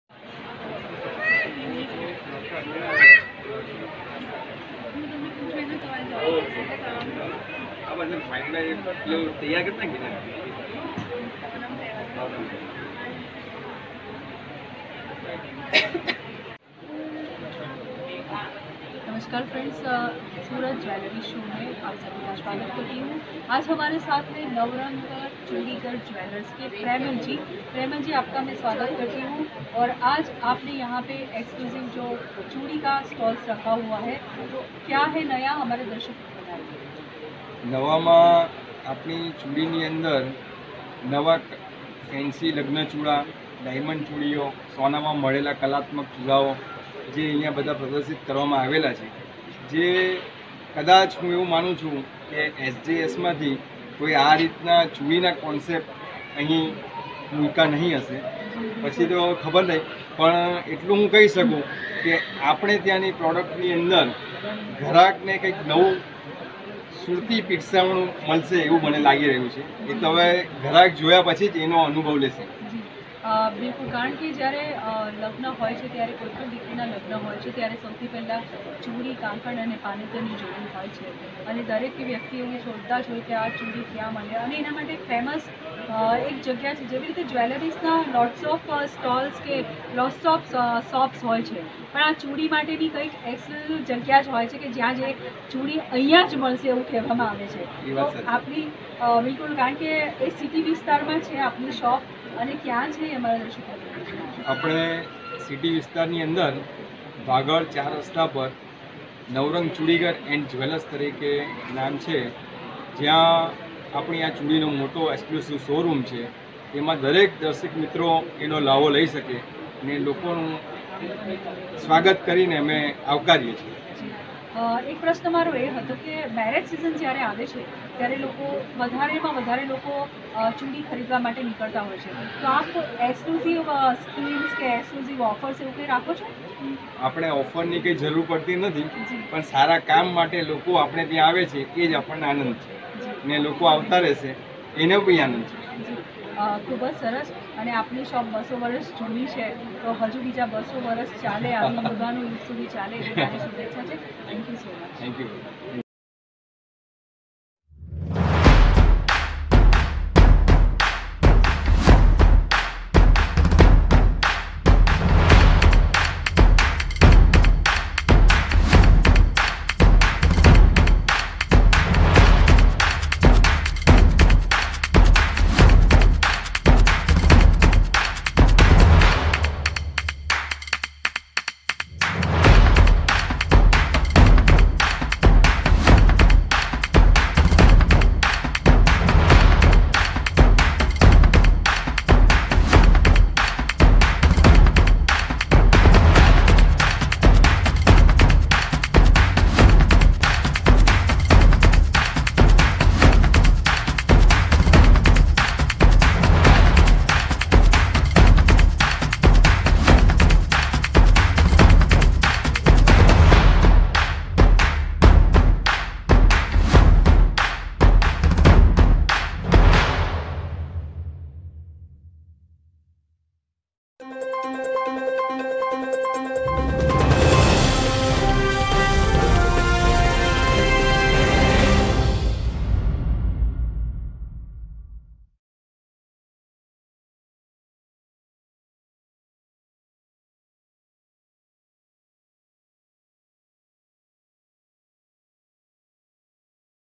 INTERVIEW OF NAVRANG CHUDIGHAR SJS SURAT JWELLERY SHOW AT INDOOR STADIUM 1 sep 2018
EXOTIC WEB MEDIA
INTERVIEW OF NAVRANG CHUDIGHAR
 SJS SURAT JWELLERY SHOW
AT INDOOR STADIUM
TODAY IS LAST DAY SO DO VISIT BEAUTIFUL WOMENS